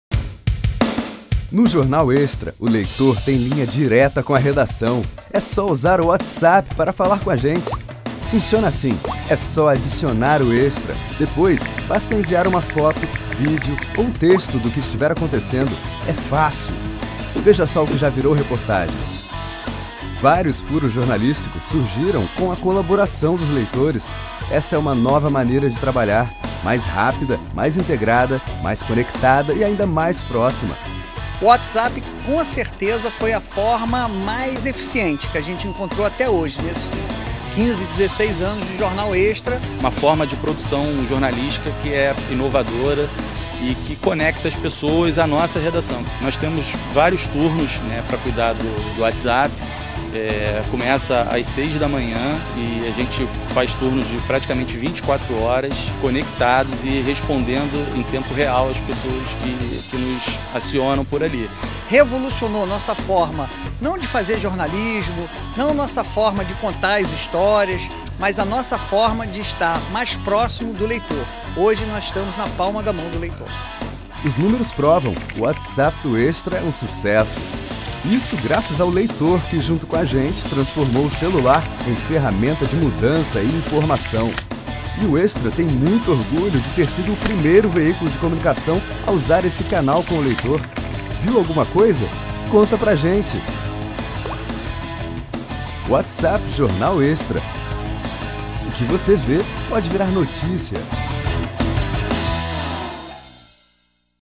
WhatsApp Jornal Extra
O Jornal Extra foi o primeiro veículo de comunicação a usar o WhatsApp para ouvir o leitor. É uma linha direta com a redação que permite o envio de mensagens de texto, áudio, vídeos e fotos. O WhatsApp do Jornal Extra já recebeu mais de 1 milhão de mensagens e furos de reportagens em todo o Brasil. Viu alguma coisa? Conta pra gente.